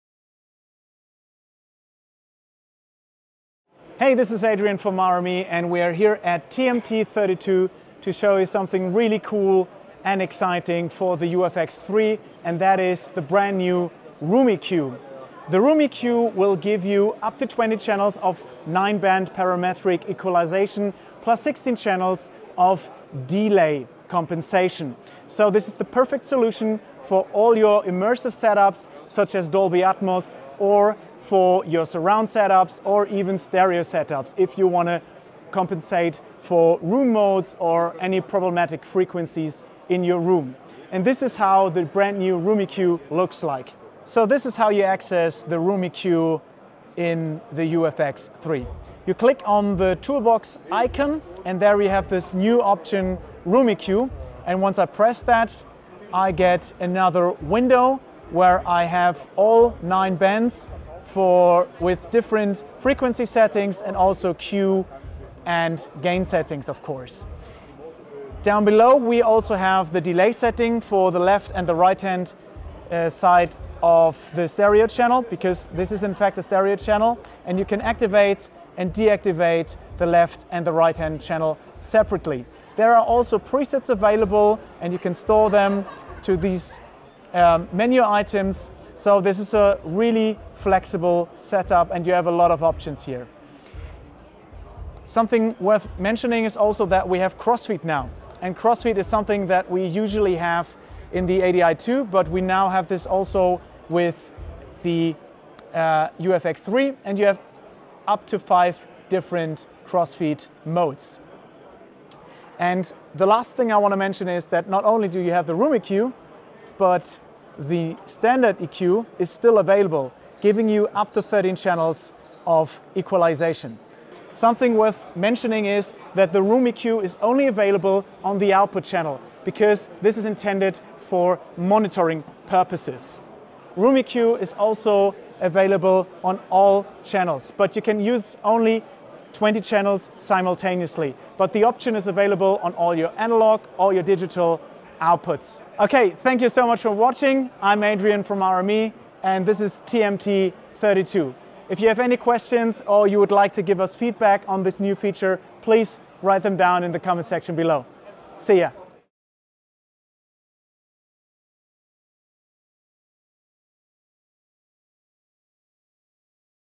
Immersive Audio comes to the Fireface UFX III with the new Room EQ (Preview)
The upcoming new Room EQ in TotalMix FX will give you up to 20 channels of 9 band Parametric Equalization, plus 16 channels of delay compensation. The perfect solution for all your Immersive Audio setups.

00:00 - Intro
00:12 - Room EQ
00:43 - Overview
01:34 - Crossfeed
02:25 - Outro